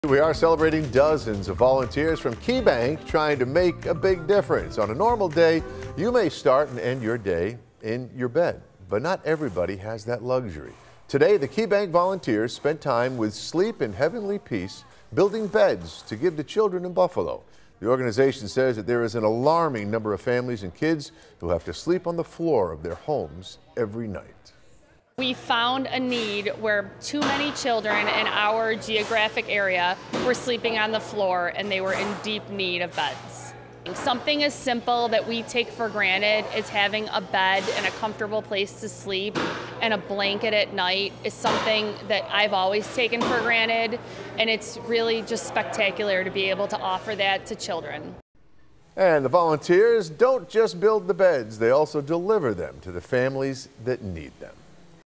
Keybank teammates give back by volunteering on annual Make the Difference Day
Keybank volunteers spent the day volunteering at different organizations including Sleep in Heavenly Peace.